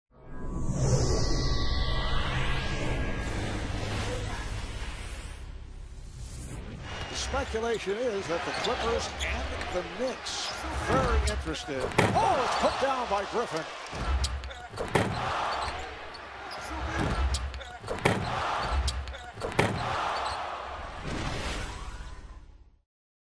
Blake Griffin grabs it and throws it down for dunk of the night
Blake Griffin grabs it with the left and throws it down with both.